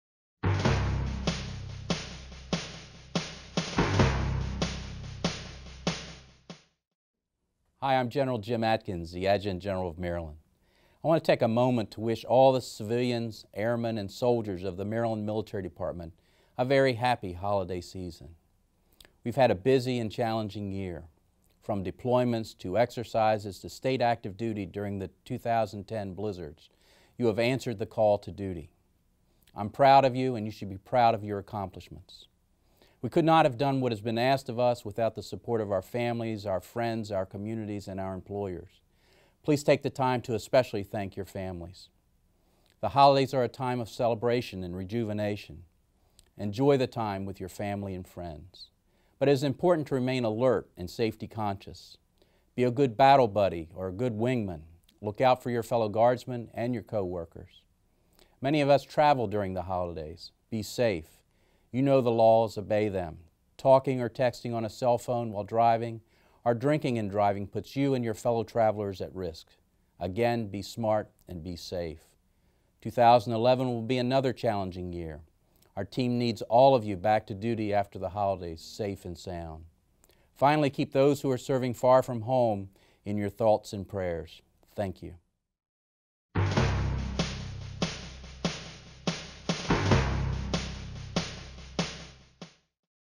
Holiday Safety Message
Holiday safety message from Brig. Gen. James A. Adkins, the adjutant general of Maryland, to all the Soldiers, Airmen and civilians of the Maryland Military Department.

We have had a busy and challenging year. From deployments, exercises and state active duty during the 2010 blizzards you have answered the call to duty.

I am proud of you and you should be proud of your accomplishments.

We could not have done all that was asked of us without the support of our Families, our communities and our employers. Please take the time to thank your Families.

The holidays are a time of celebration and rejuvenation. Enjoy the time with your Family and friends. It is important to remain alert and safety-conscious. Be a good battle buddy or a good wingman. Look out for your fellow Guardsmen and co-workers.

Many of us travel during the holidays. Be safe. You know the laws. Obey them.
Talking or texting on a cell phone while driving or drinking and driving puts you and your fellow travelers at risk. Again, be smart, be safe.

Next year will be another challenging year. Our team needs you all back to duty safe and sound after the holidays.

Finally, keep those who are serving away from home in your thoughts and prayers. Thank you.